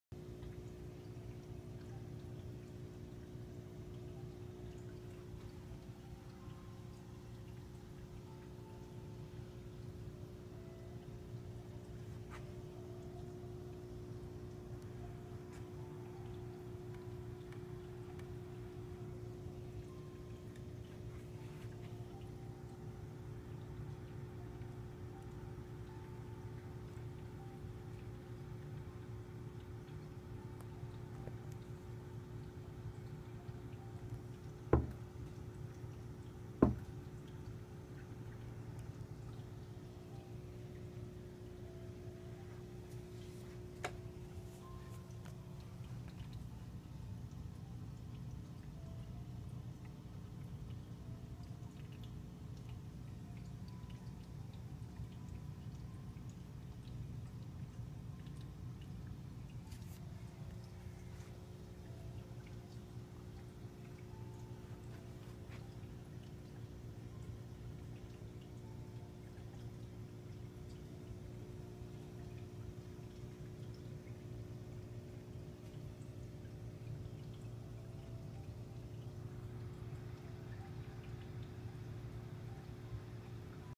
My aquarium mix of adult cichlid fish and fry together
Almost a month old baby convicts and even Oscar or Jack Dempsey dares to eat them. They got severely beaten by male convict and now stay away.